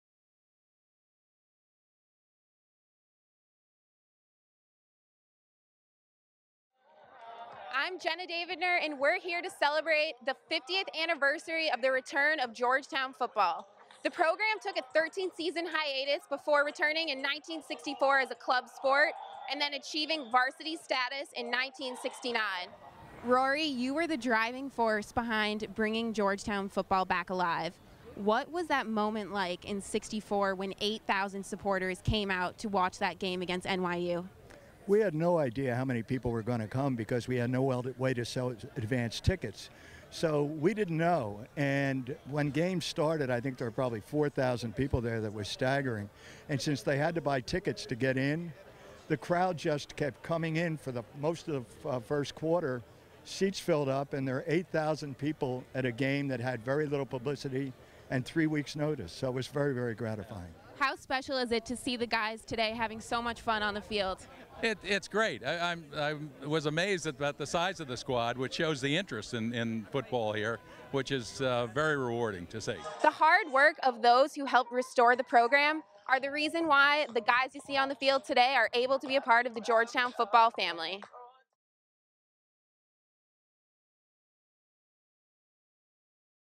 50 Years of Georgetown Football
Georgetown Buzz celebrates the 50th Anniversary of the return of football to the Georgetown.